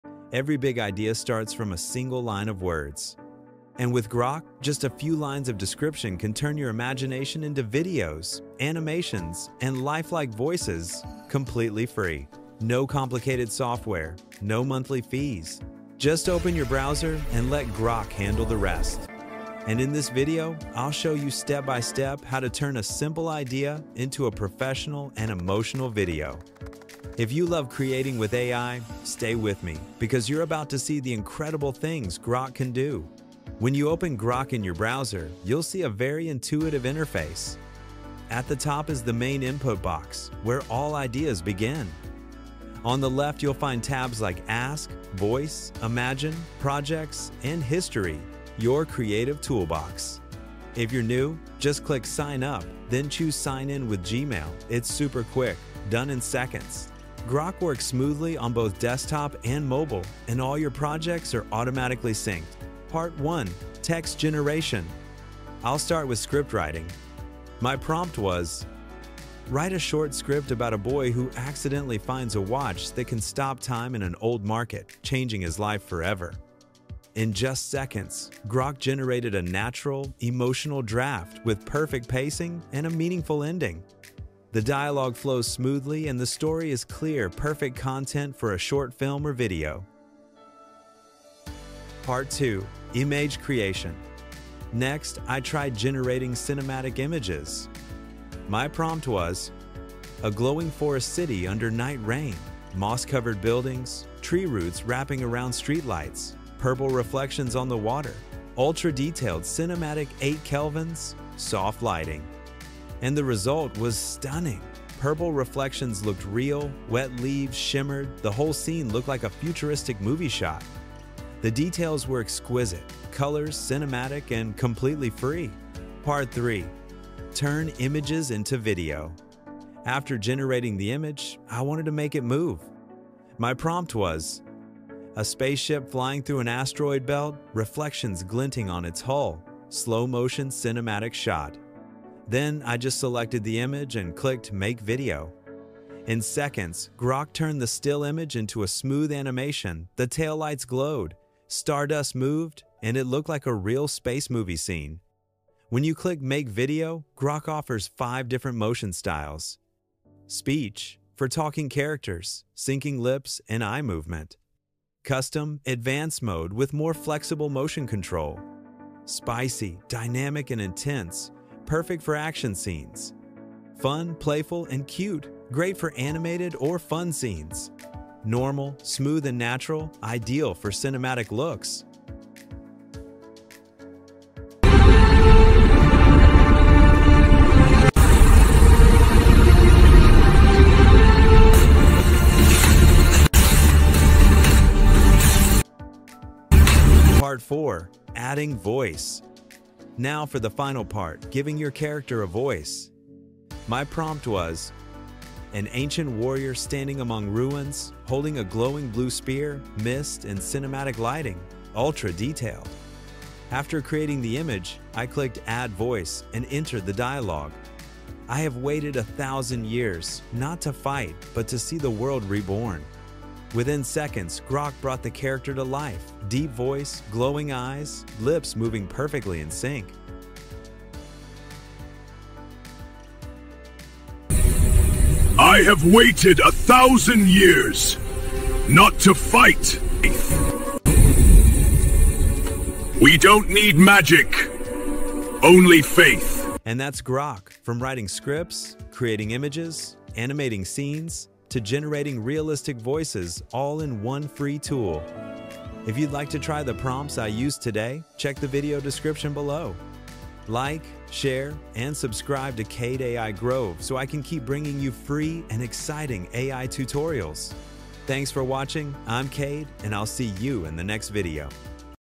Grok AI Review – Free, Unlimited, and Absolutely Mind-Blowing
Welcome to Cade AI Grove! In this video, we dive deep into Grok AI — the free and unlimited creative tool that’s changing the way we make videos, images, and voices. 🚀 With just a few lines of description, Grok turns your imagination into cinematic visuals, lifelike animations, and natural voices — all within your browser, no downloads or monthly fees.

We’ll explore how to write scripts, generate images, and animate your scenes using Grok’s simple interface. Whether you’re a content creator, filmmaker, or just love AI art, this tutorial will show you how to create professional results effortlessly.

Experience the future of creativity with Grok AI – Free, Unlimited, and Absolutely Mind-Blowing!